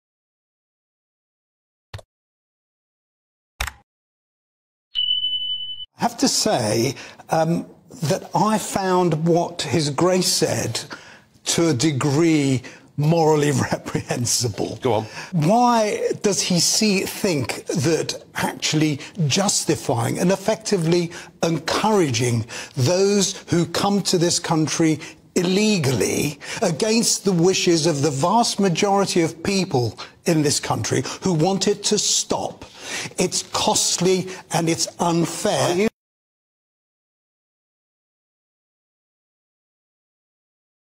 The Archbishop of Canterbury is out of touch on immigration
Your Grace, questioning the morality of those people who favour the government’s efforts to stop illegal Channel crossings is a truly shocking slur.

It also encourages migrants to risk or even lose their lives and effectively aids criminals to make more money... 😲